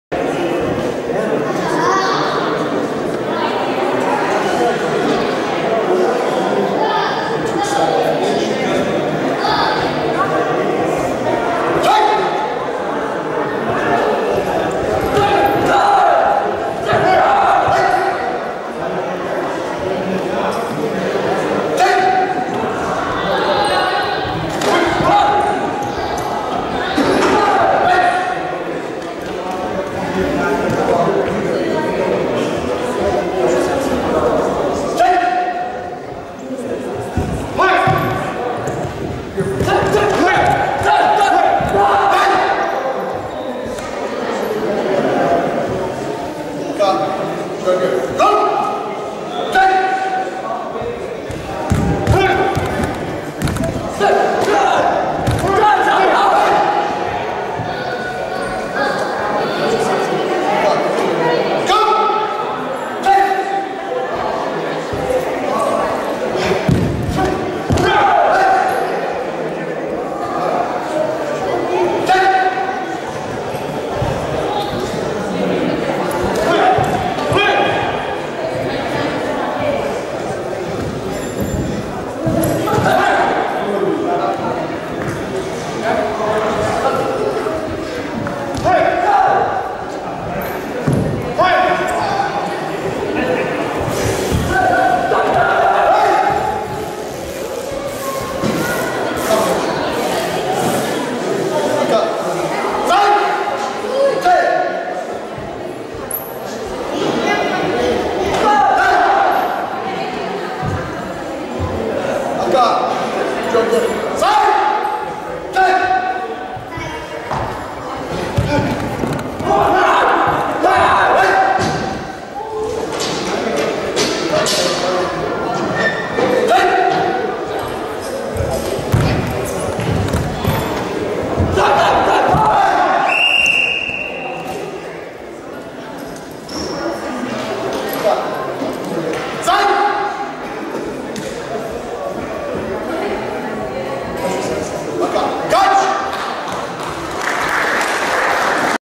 wrka kyoso cup 2017 11 04 rg2 division m08 17yrs+ male advanced kumite 04d
2017 WRKA Kyōsō Cup Karate Championships

04 November 2017

Ring2: Division M08 17yrs+ Male Advanced Kumite